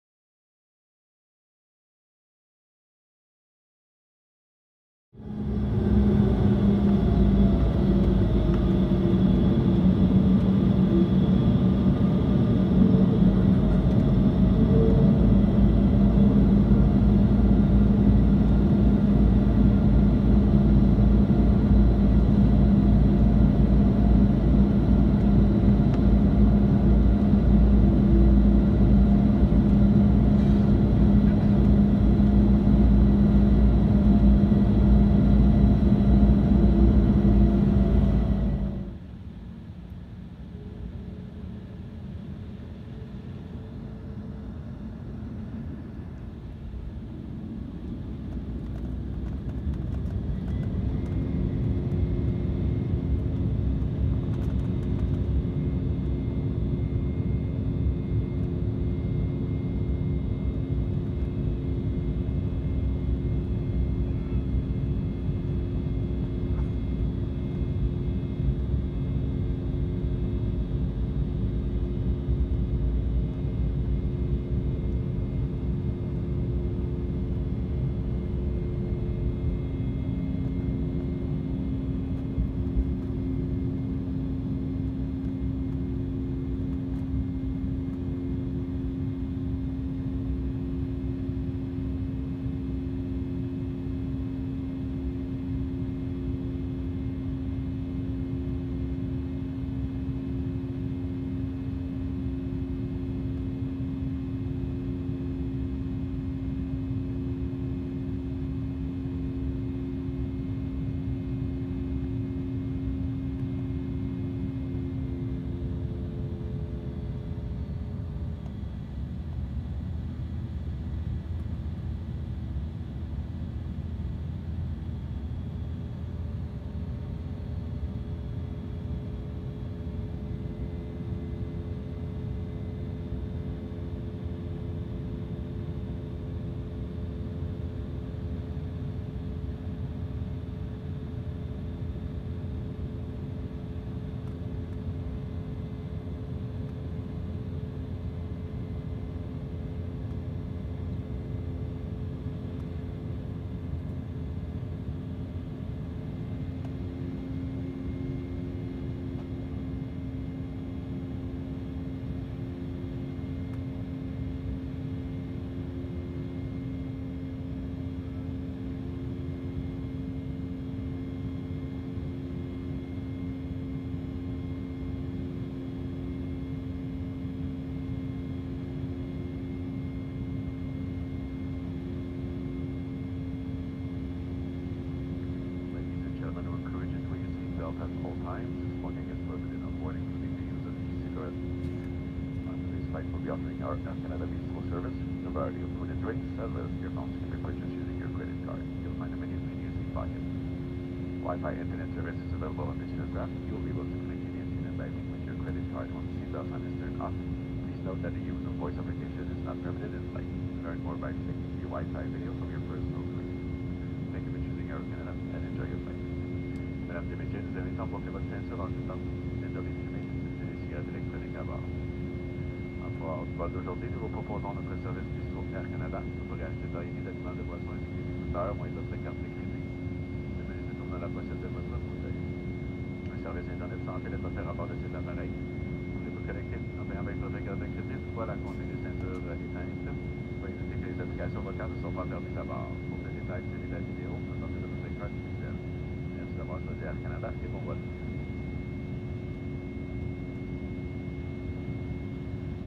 Air Canada Embraer190 Takeoff from Regina
This is my last ever Takeoff from YQR on the Embraer 190 as all of these planes over the summer will be full retired and sold off. It was always a pleasure on the E190, and still have one flight left tomorrow (this being posted Feb 22/23). Next time I leave YQR it will probably be on an Airbus A319/20. And RIP to the E-190. Great plane.